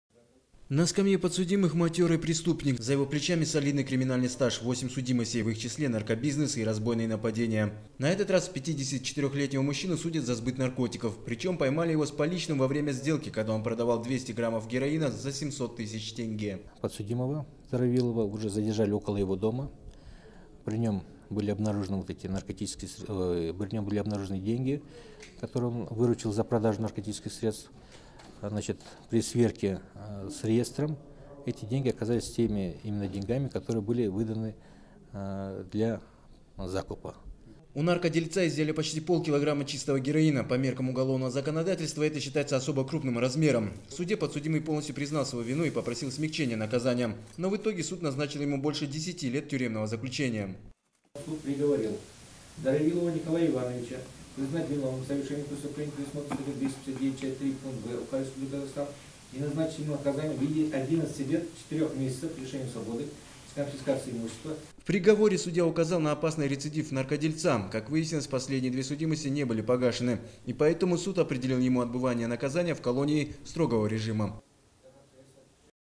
Наркобарон в Шымкенте получил 11 лет строгого режима
В специализированном уголовном суде завершился процесс над наркодельцом.
На скамье подсудимых матёрый преступник. За его плечами солидный криминальный стаж - 8 судимостей, в их числе наркобизнес и разбойные нападения. На этот раз 54-летнего мужчину судят за сбыт наркотиков. Причём поймали его с поличным во время сделки, когда он продавал 200 граммов героина за 700 тысяч тенге.
Абинур Карабаев, судья межрайонного специализированного суда по уголовным делам: "Был оперативный закуп, гражданка передала ему меченые купюры 700 тыс тенге, он передал 191 грамм героина, после задержания он добровольно сдал и другую часть наркотиков, хранившиеся в доме его приятеля".